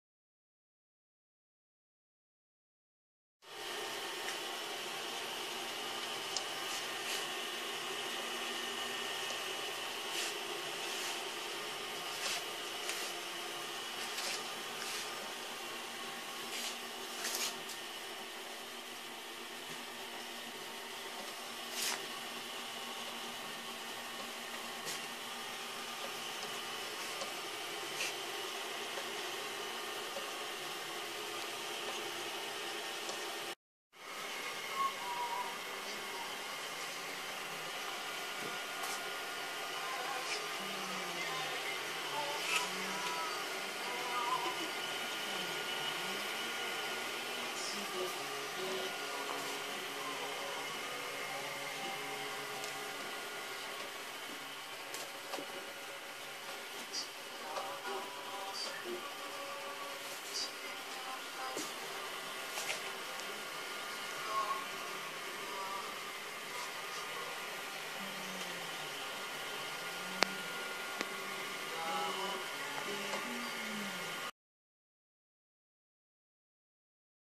3D printed Lamp 활용 예
I printed lamp with 3D printer and then assemble it with real bulb and electric cable. You can see the example to utilize in the real world.
3D프린터로 출력한 램프를 활용한 예입니다. 실제 전구와 전선을 연결해서 활용해봤습니다.